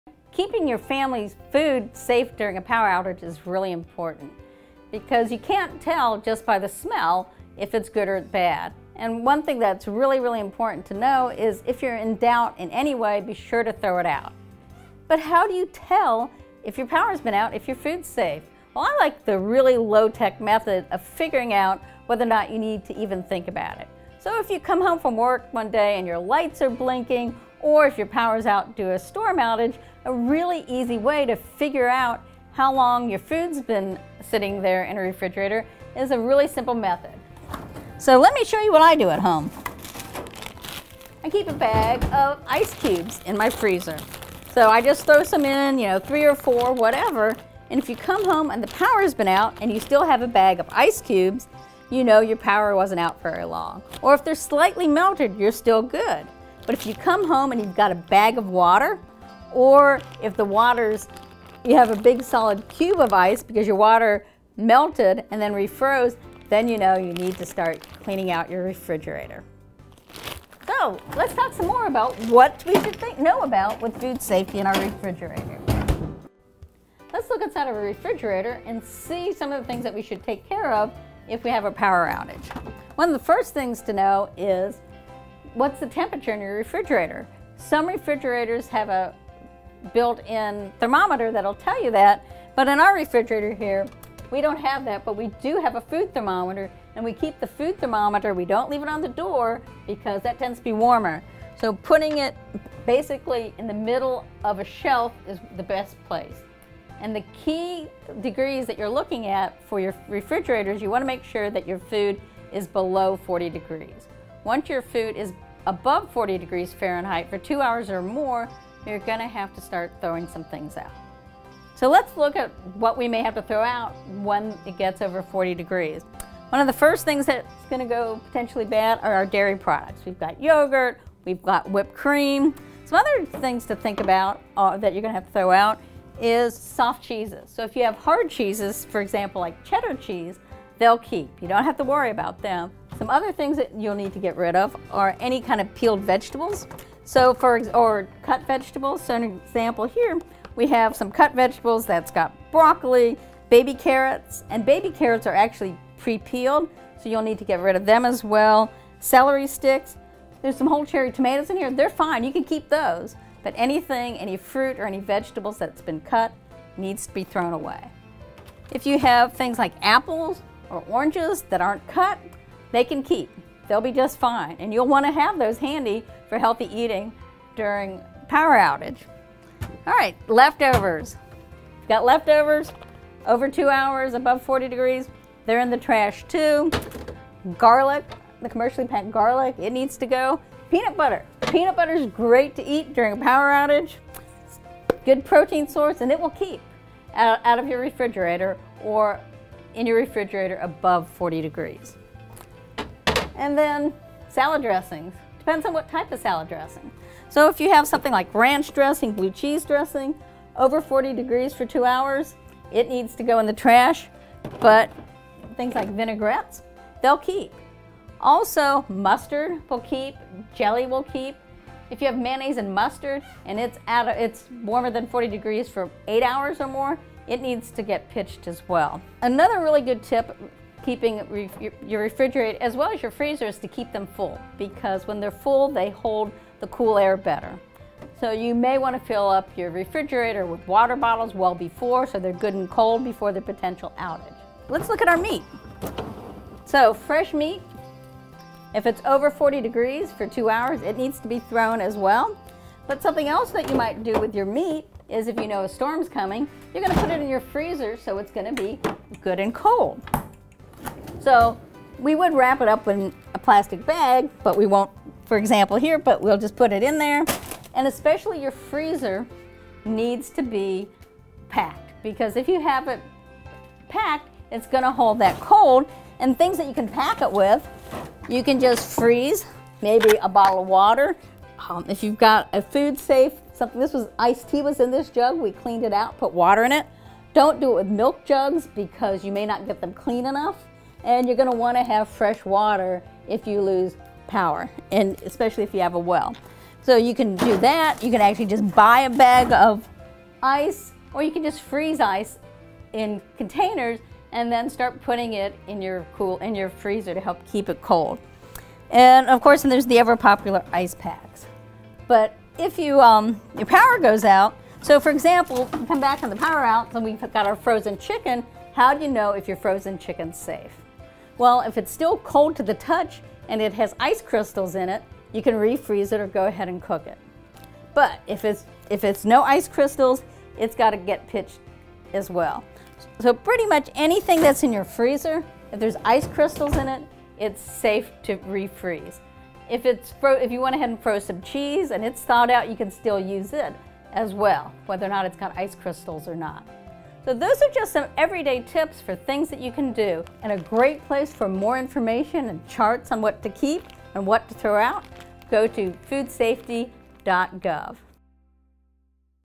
Food Safety After a Power Outage
What food can I eat after a disaster?  How long will it be good for?  What should I eat first?  This video will highlight food safety after a disaster.  You may not have to throw out all of your food.  In fact, if you are food smart, you may be able to eat most of the food you have in your freezer and refrigerator if managed properly.